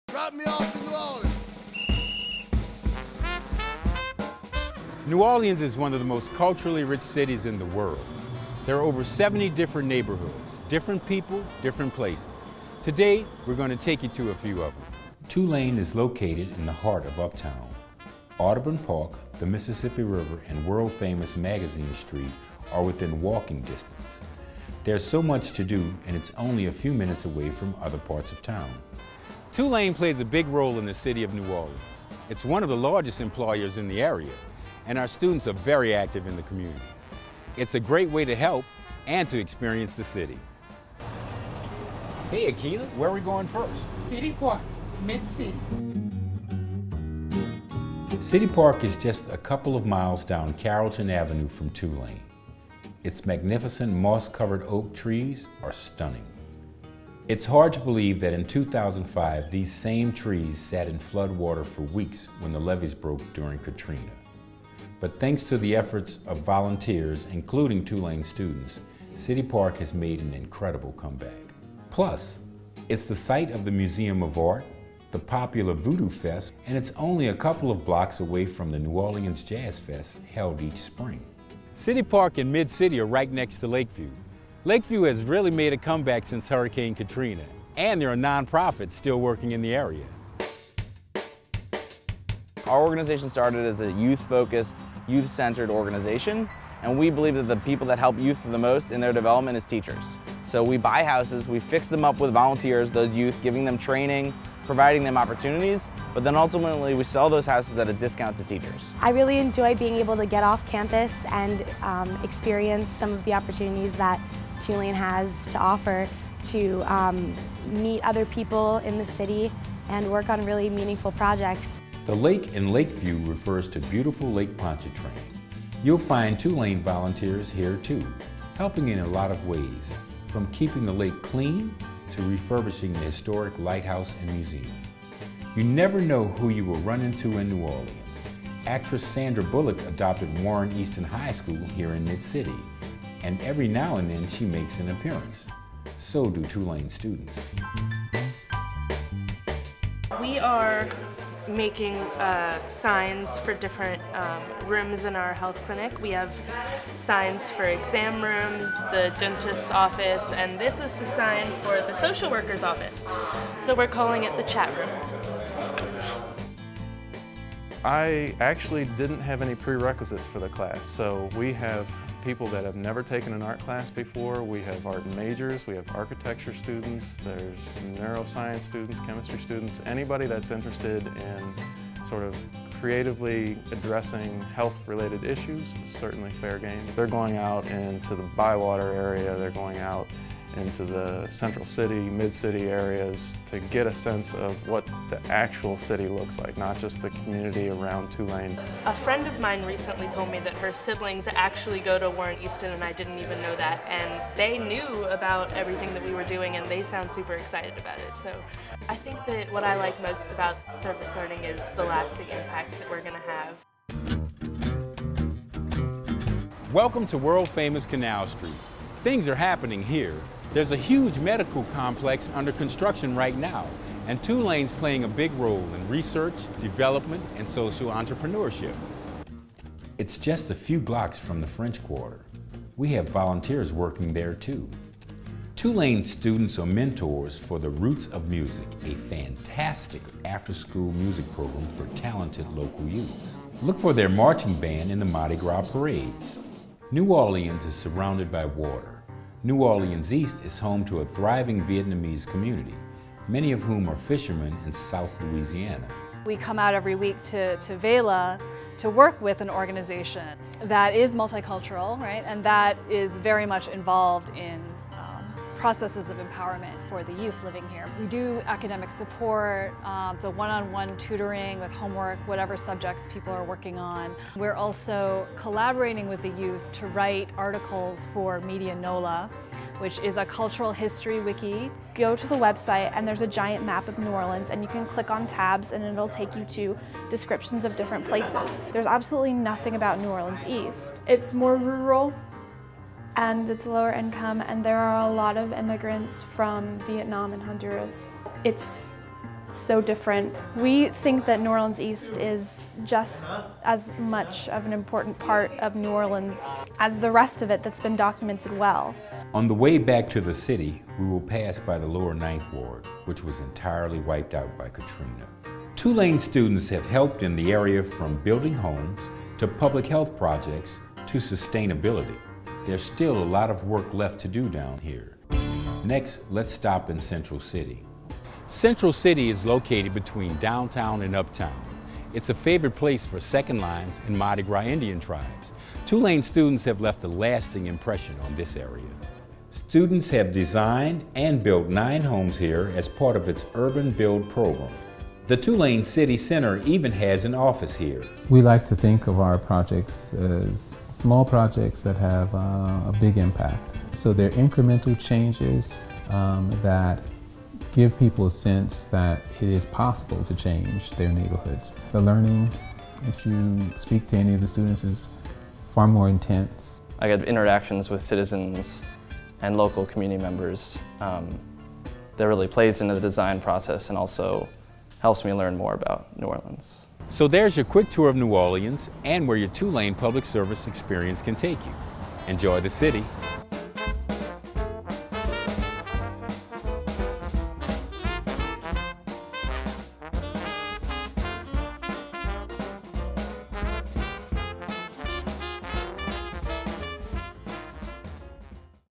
TU & You in the Community
A signature part of the Tulane undergraduate experience is getting involved in public service. Through unique partnerships between the university and community organizations, students are able to interact with people they would not otherwise meet while gaining a better understanding of life in New Orleans beyond the Tulane campus.